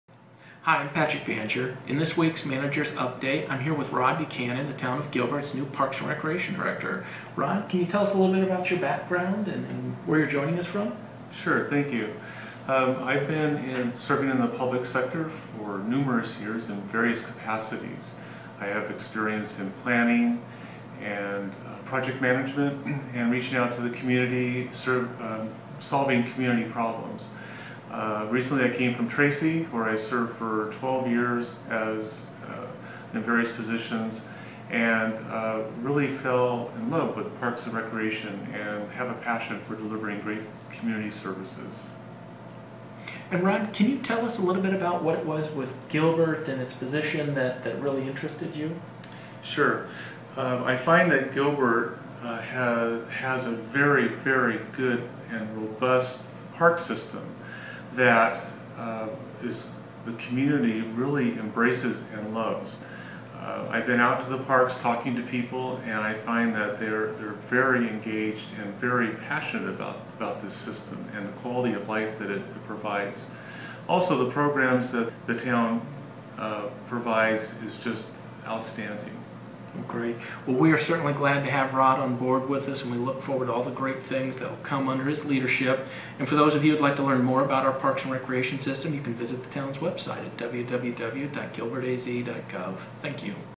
Manager's Update: Gilbert's New Parks & Rec. Director
In this weeks Manager's Update, Town Manager Patrick Banger speaks with Rod Buchanan, the new Parks & Recreation Director for the Town of Gilbert.To find out more about Parks & Rec. in Gilbert, Arizona